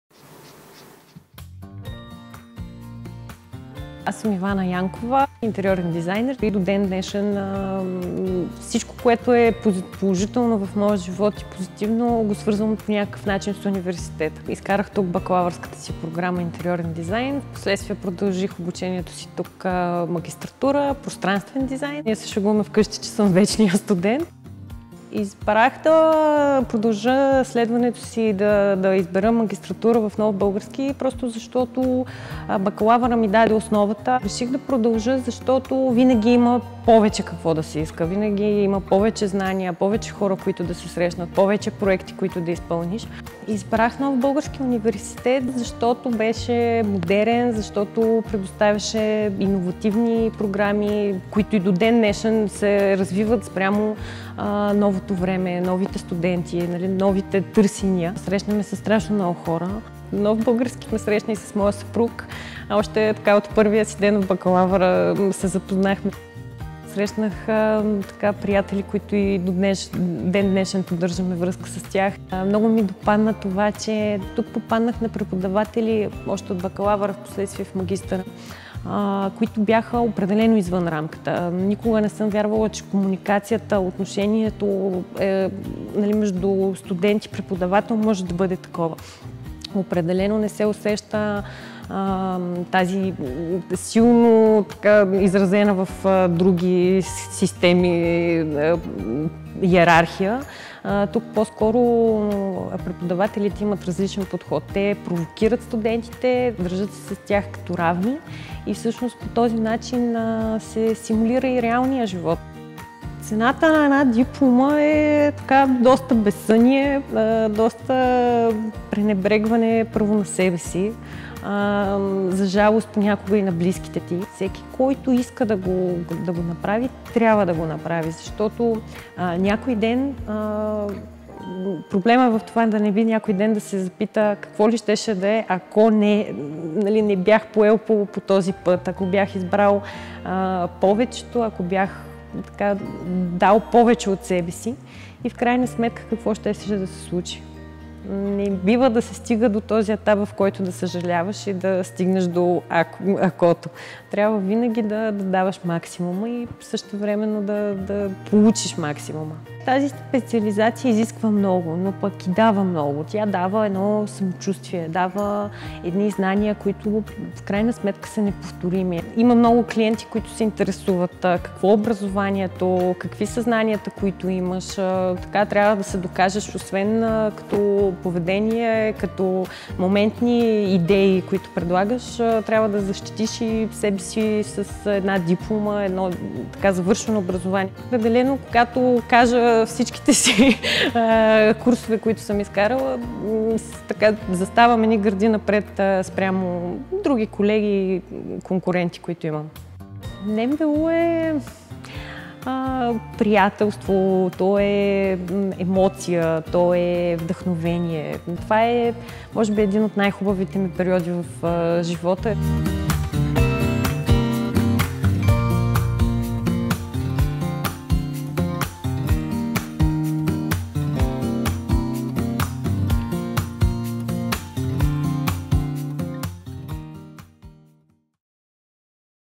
Ивана Янкова - историята на един успешен магистър на НБУ
Да срещнеш любовта в НБУ!

„Нов български университет е знание, емоция, приятелство и вдъхновение.“ Така започва разказът за студентските си години в Нов български университет Ивана Янкова. Завършва бакалавърска програма „Интериорен дизайн“, а след това и магистратура „Пространствен дизайн“. Още в първите си дни като студент среща любовта на живота си. А днес е омъжена за него.

Работи като интериорен дизайнер, а опитът й с клиенти доказва колко важна е професионалната квалификация и дипломата. Ивана печели конкурси в страната и чужбина с интериорни проекти на обществени и жилищни сгради. От 2012 г. има собствена школа за изкуство и дизайн.

Спомня си безсънните нощи над книгите и проектите за Университета, но и подкрепата на нейните преподаватели и колеги. Казва, че това са едни от най-хубавите моменти в живота й. И е категорична, че е взела правилното решение – да инвестира в себе си.

Сега е моментът да инвестирате в себе си и вие – с магистърските програми на Нов български университет!

Излезте от рамката!

❗️Срокът за подаване на документи за есенния прием на 2020 г. е до 25 септември.